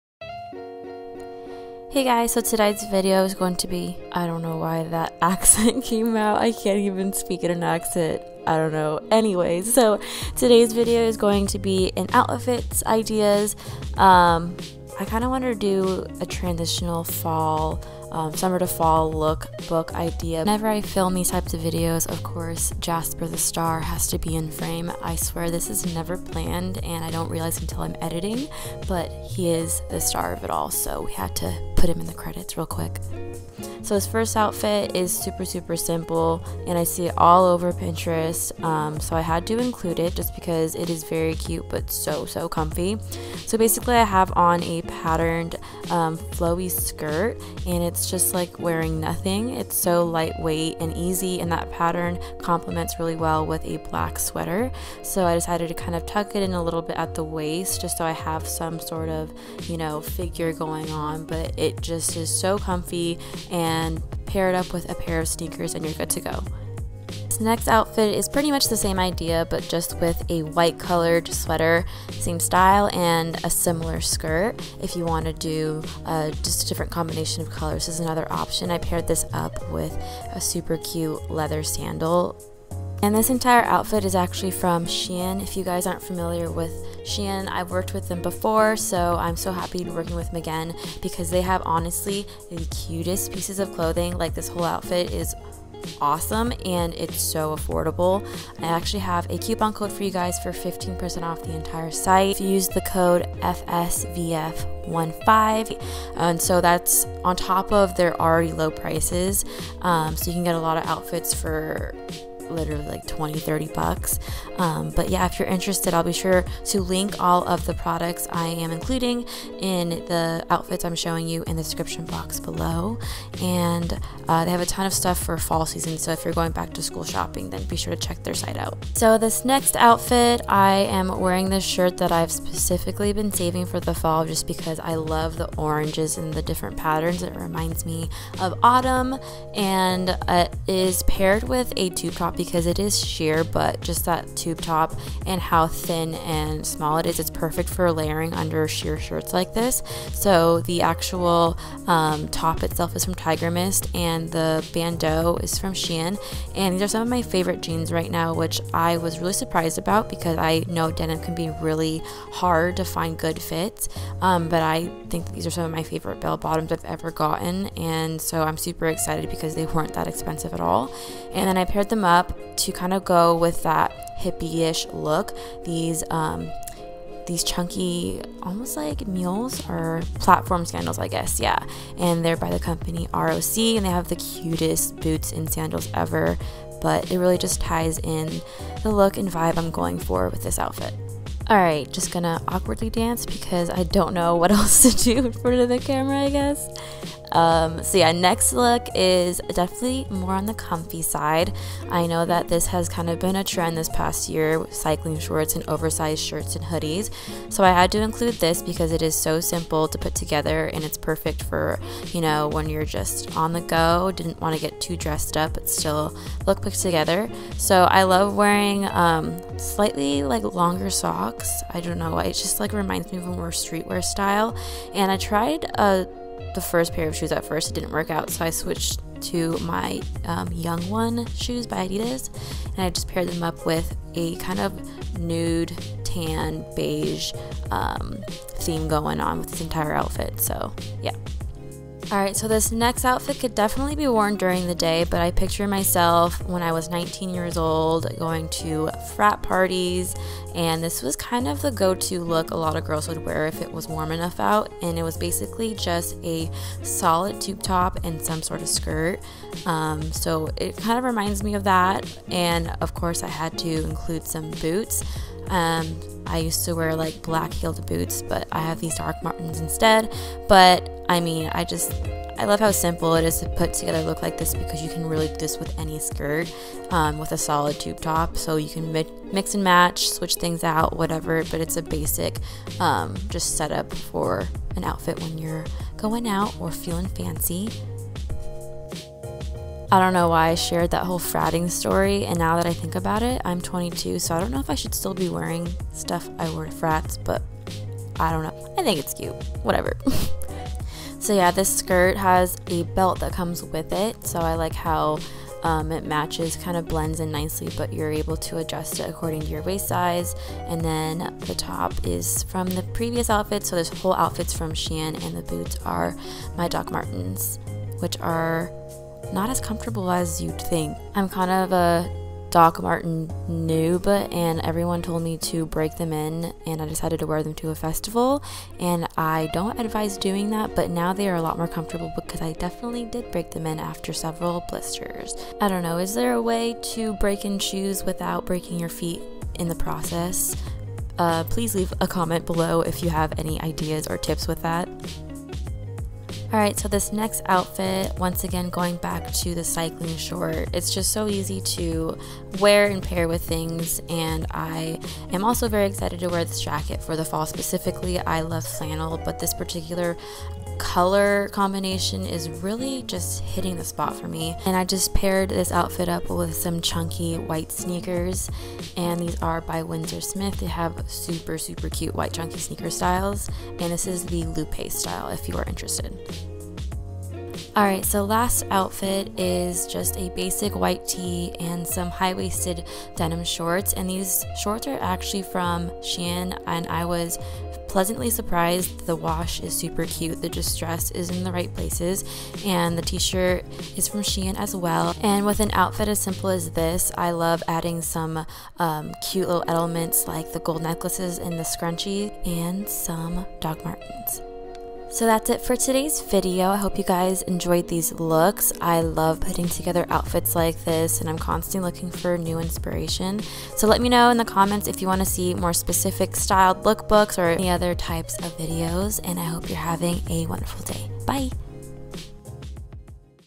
cute but comfy outfit ideas | viviannnv
hi loves! hope you like these outfits i put together, let me know if you want more styling videos or hauls or whateva!!

shoes: windsor smith lupe sneaker

OUTFIT 3:
Top: Tigermist - KAHLO TOP
ROC platform sandalS

OUTFIT 4:
Sweatshirt: White Fox
Shoes: Adidas Yung 1

Jacket- Tigermist 'Brooklyn Jacket'
Shoes- Windsor Smith 'Lupe'